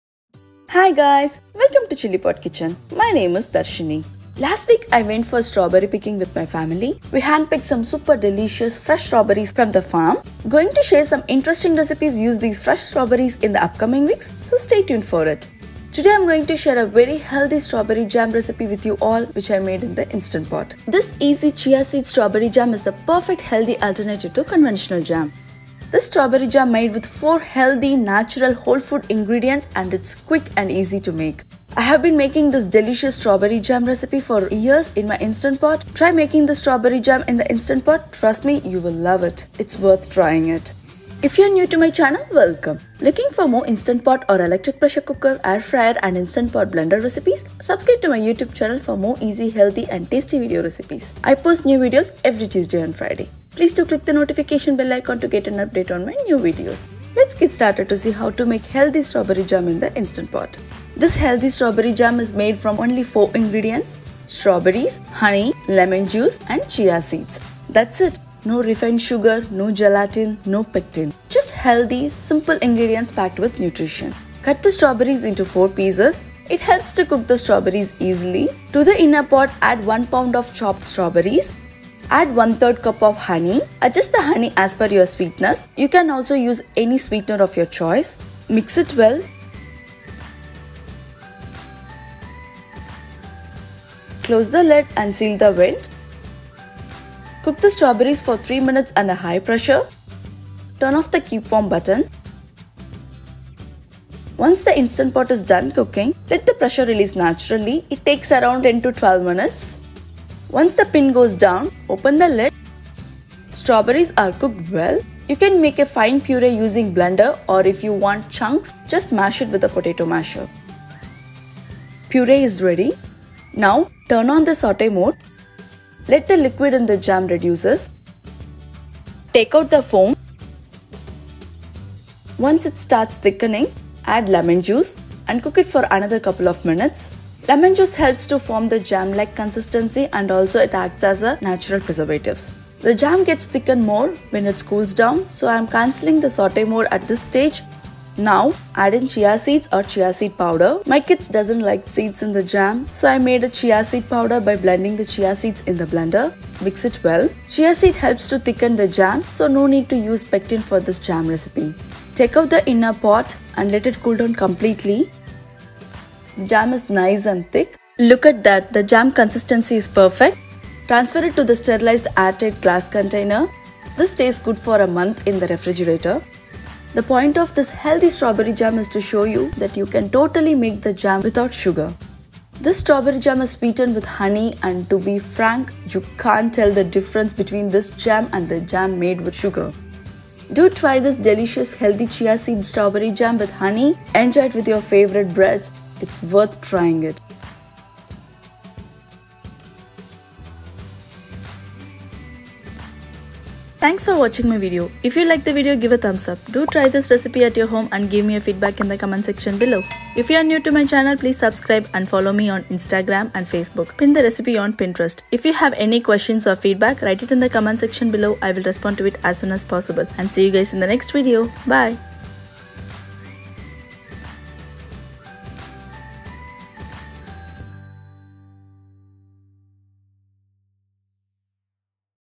Instant Pot Strawberry Jam | No Sugar| No Pectin | Healthy Strawberry Jam With Chia Seeds & Honey
This Easy Chia Seed Strawberry Jam with honey made in the instant pot is the perfect healthy alternative to conventional jam! It's made with 4 healthy, natural, whole-food ingredients and it's quick and easy to make!

If you make this recipe, share your food photo with chillipotkitchen
@chillipotkitchen on Facebook or Instagram, so we can see it and
others can enjoy it too! Thank you!

Cook Time- 3 minutes + 15 minutes sauté
Cooling Time- 1 hour
Total Time- 1.30 hour
Yields- 7 Oz (approximately)
Instant Pot Model- 3 qt DUO (USA Buy (India Buy
Hand Blender- (USA (India

Nutrition Facts: (approximately)
Serving Size: 1 Tbsp
Calories: 32kcal | Carbohydrates: 3g | Protein: 0.5g | Fat: 0.4g | Saturated Fat: 0g | Sodium: 0.2mg | Potassium: 32mg | Fiber: 1.7g | Sugar:5g | Calcium: 10mg | Iron: o.3mg

Ingredients:
Strawberry- 1 lb/450 gms
Honey- 1/3 to 1/2 cup (depends on your level of sweetness)
Chia seed Powder- 2 Tbsp (USA (India

Method:
* To a pot add strawberries and honey. Mix it. Close the lid and seal the vent.
* Cook it for 3 minutes and do natural pressure release (NPR).
* Make a purée, and sauté it to thicken.
* Once it starts thickening add in lemon juice and cook it for another couple of minutes.
* Cancel the sauté mode and then add chia seed powder, mix it well.
* Store it in an airtight container in the refrigerator stays good for a month.

Please leave your feedback and comments in the comment section below!

means that if you click on one of the product links and make a